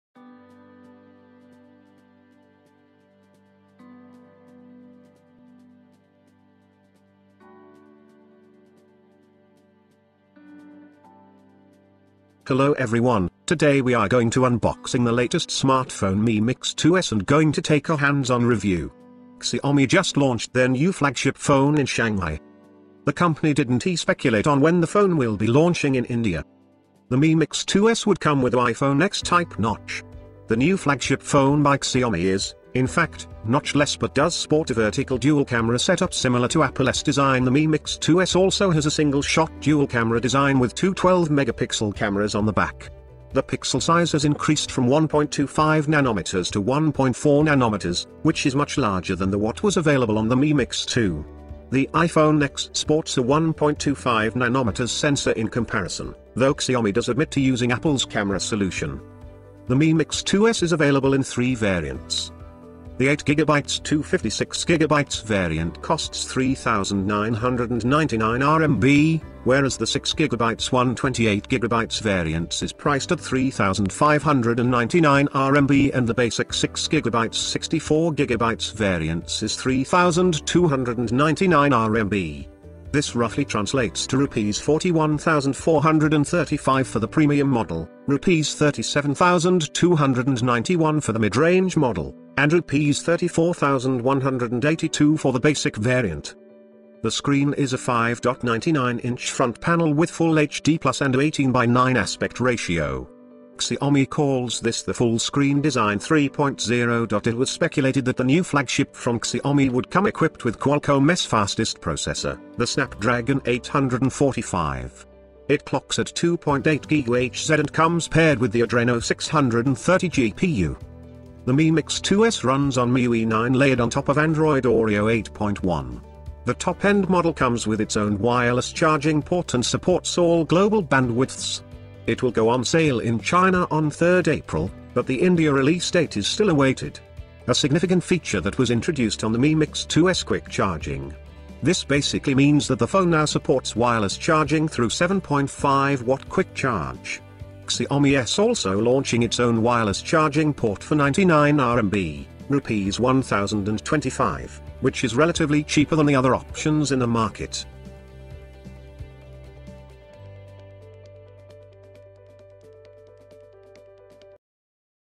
Mi Mix 2S Unboxing and Hands On {Enter21st.com}
In this video We are going to show you unboxing and quick review of Mi Mix 2s. Check price, release date, full specification of Xiaomi Mi Mix 2s.